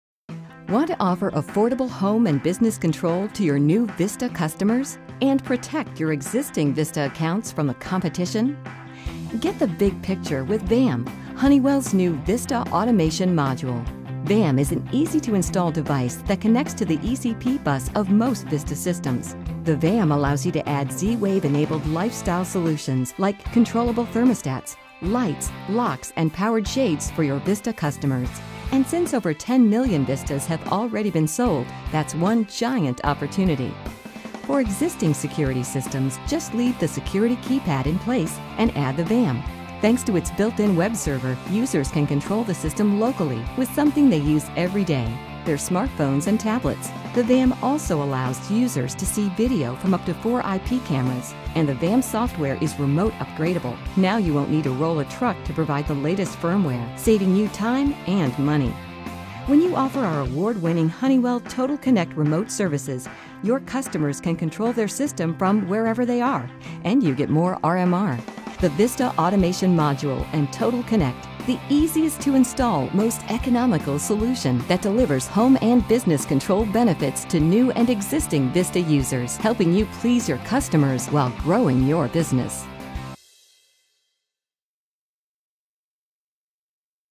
Vista Home Automation Module VAM
Learn more about the affordable VAM module for home and business control for existing Honeywell VISTA security systems. Now you can easily add home automation to old Vista alarm systems. Pay less, Go GEOARM.